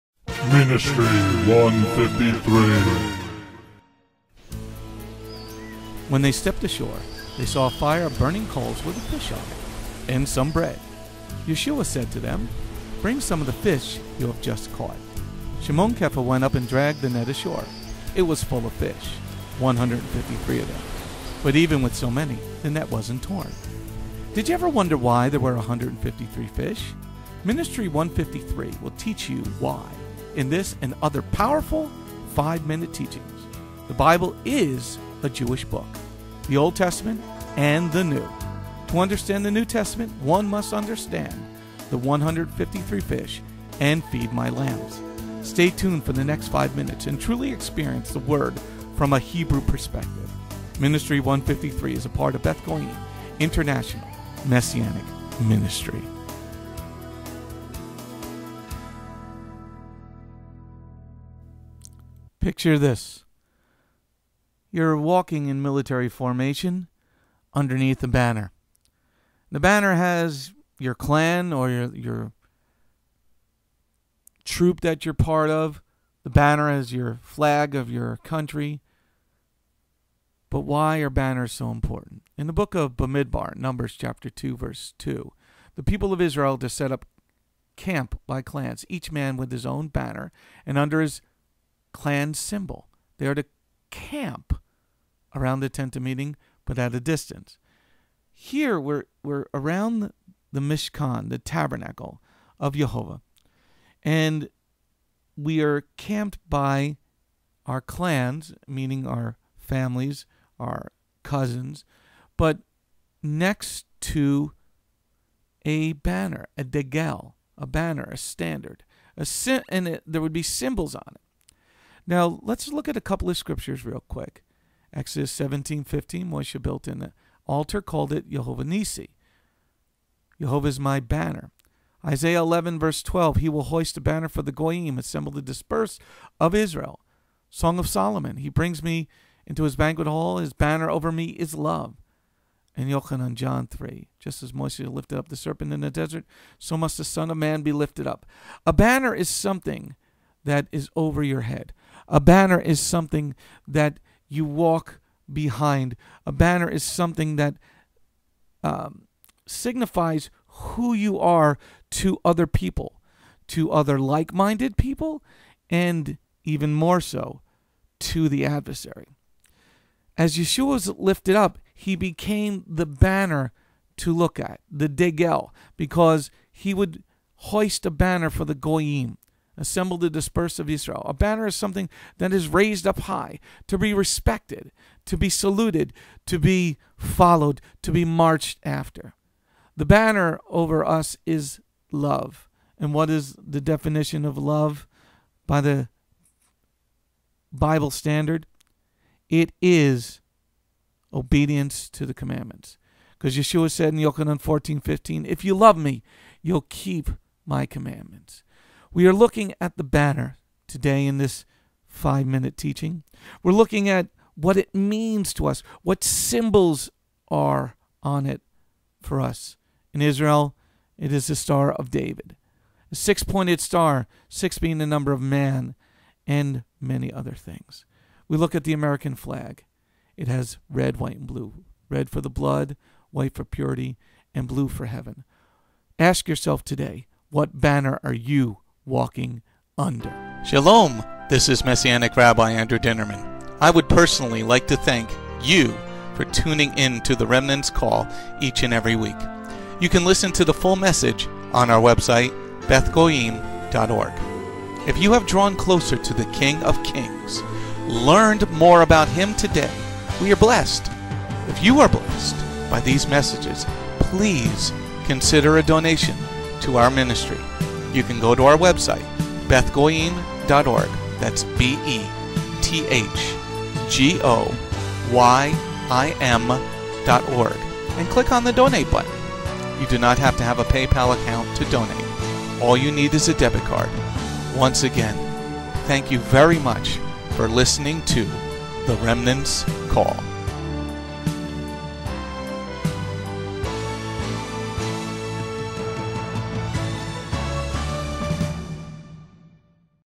Min153P034 2
Num 2:1  Adonai said to Moshe and Aharon,
Num 2:2  "The people of Isra'el are to set up camp by clans, each man with his own banner and under his clan's symbol; they are to camp around the tent of meeting, but at a distance.

Banner H1714 דּגל  degel  BDB Definition:banner, standard
Symbol H226  אות  'ôth  BDB Definition:  sign, signal, a distinguishing mark, banner, remembrance, miraculous sign, omen, warning, token, ensign, standard, miracle, proof

Law of 1st ref.
Exo_17:15  Moshe built an altar, called it Adonai Nissi [Adonai is my banner/miracle],

Banner tie together with the disgusting isis flag and the gay flag

Isa 11:12  He will hoist a banner for the Goyim, assemble the dispersed of Isra'el, and gather the scattered of Y'hudah from the four corners of the earth.

Son 2:4  He brings me to the banquet hall; his banner over me is love.

Joh 3:14  Just as Moshe lifted up the serpent in the desert, so must the Son of Man be lifted up;